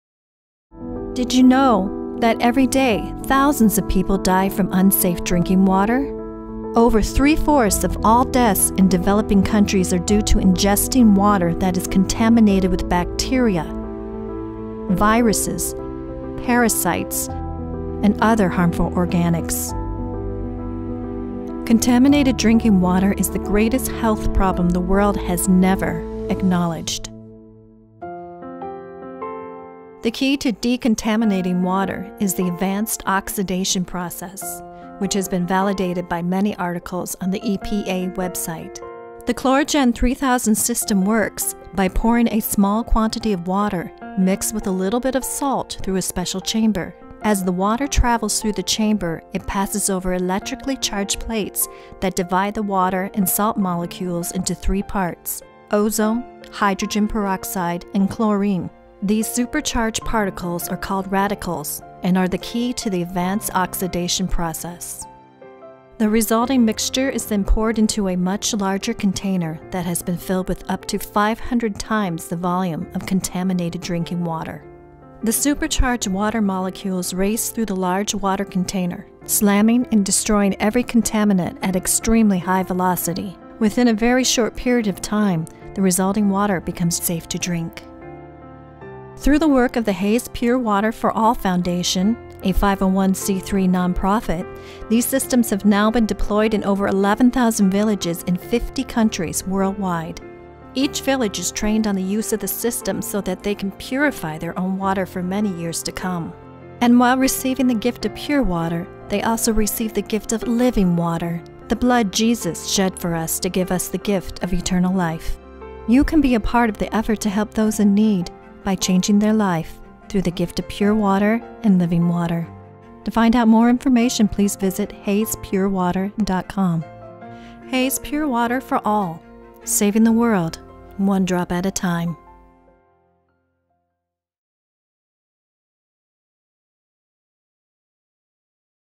Hays Pure Water For All - Saving the World One Drop At A Time
- Thousands of people die each day from unsafe drinking water. Meet John Hays, the inventor of a water purification system that has been deployed in 11,000 villages in 50 countries to make a difference.

Using his skills as an inventor, John Hays developed a water purification system that kills all 1000 bacteria, viruses and parasites known to man, that can take contaminated water and make it safe and drinkable. Based on the Advanced Oxidation Process, the KhlorGen 3000 is a small portable device that can purify water for a village of up to 5000 people for many decades.

The Hays Pure Water for All Foundation, a 501(c)3 nonprofit, has as its mission to spread these systems around the world to share Pure Water so that people can be free of diseases caused by contaminated water, and also to share Living Water (the blood Jesus shed for us to give us the gift of eternal life).

The systems are very portable, and can be powered by the included solar powered battery so they can be used in remote villages that don't have electricity. One KhlorGen 3000 system costs only $100 and can provide clean water for a village of up to 5000 people for many decades. If you would like to make a contribution of one or more systems to those in need, please for more information.

and 100% of your contribution will go directly to the Hays Pure Water Foundation.

This video was created by Lane Wyrick ) as an independent project collaboration with John Hays.